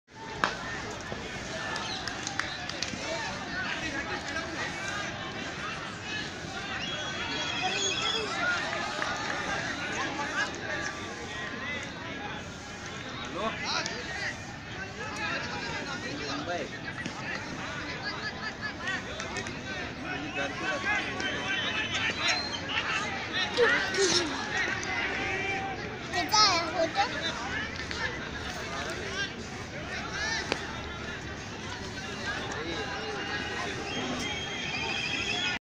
Cross Maidan (Mumbai) | Cross Maidan Morning | Cricket in Cross Maidan
During mumbai junior tournament at cross maidan on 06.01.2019

Cross_Maidan_(Mumbai)